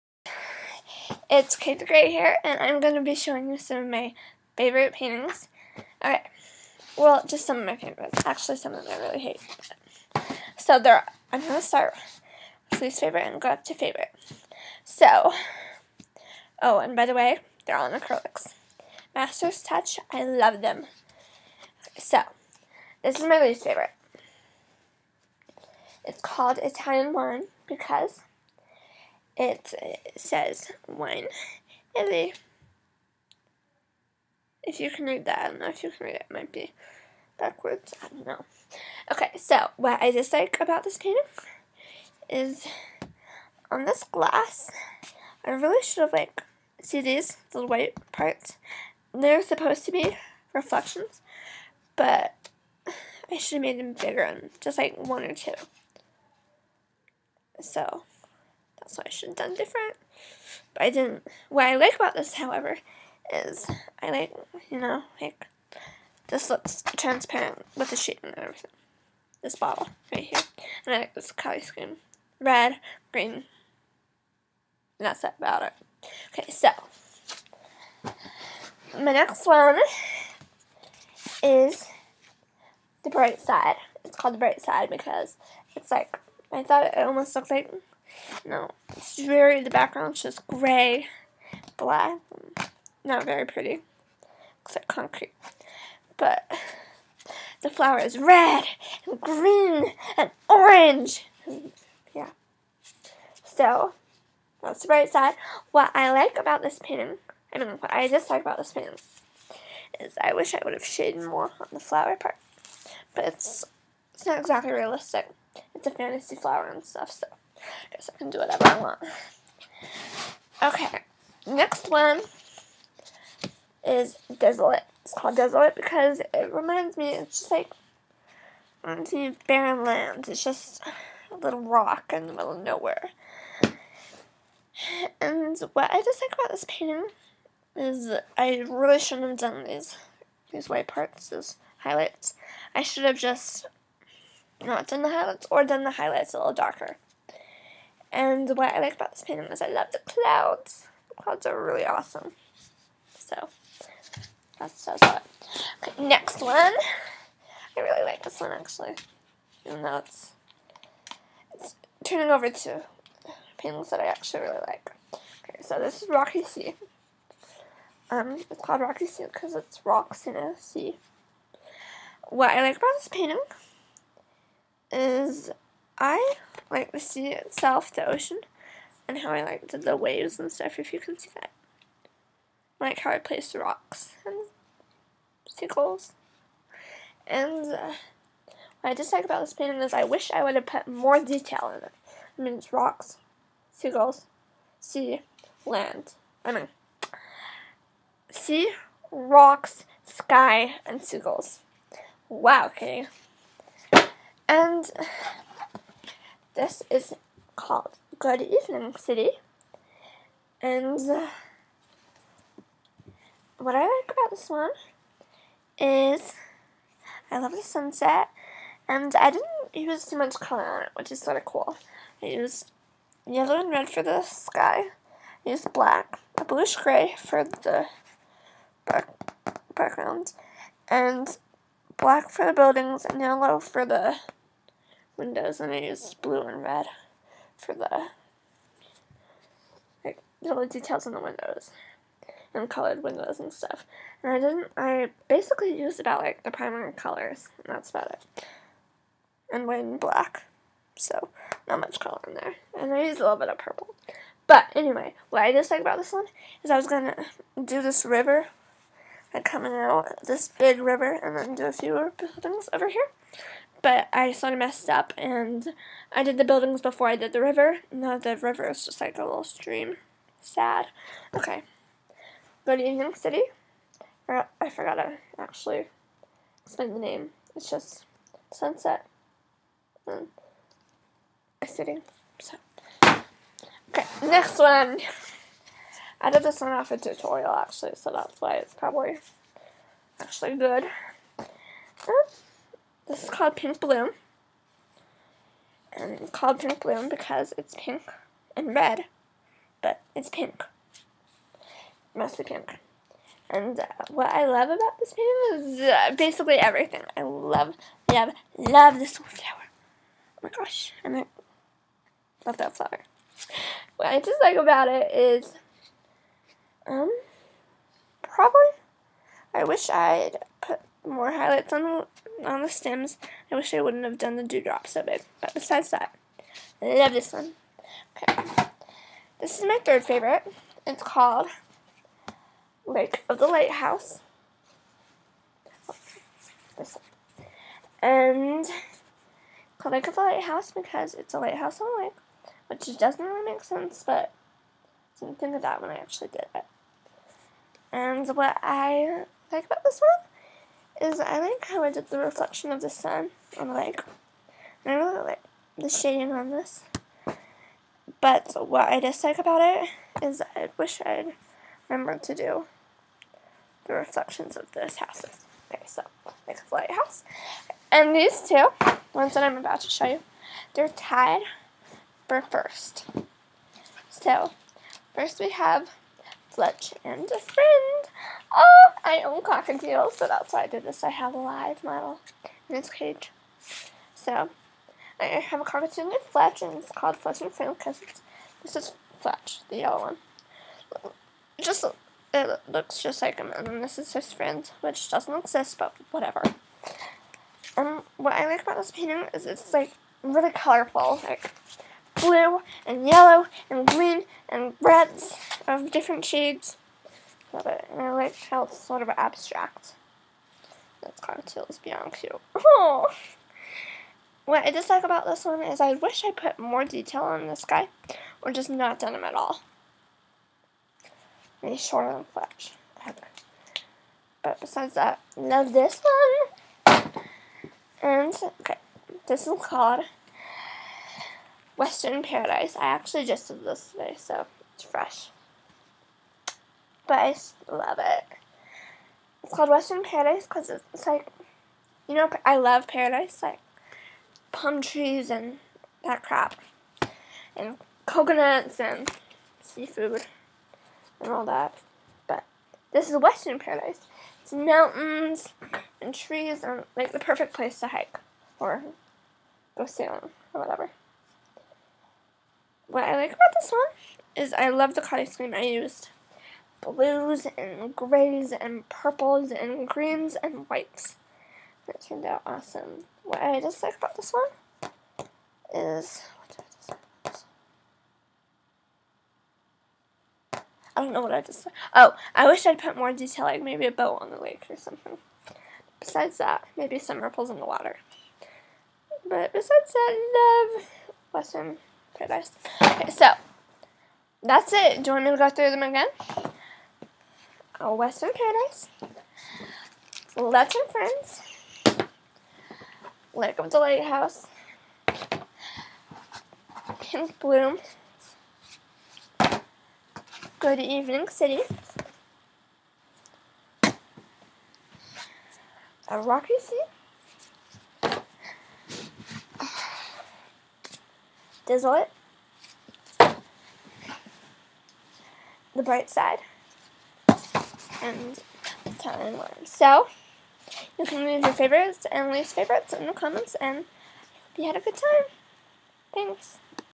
Showing off my paintings. (=
In  this video I'm showing you all my paintings from least to greatest in favoritism. I used Master's Touch Acrylic Paints in all of them and I highly recommend them for anybody, because they're good quality without the price.
9. Italian Wine
8. Bright Side
7. Desolate
6. Rocky Sea
5. Good Morning, City
4. Pink Bloom
3. Lake of the Lighthouse
2. Fletch and Friend
1. Western Paradise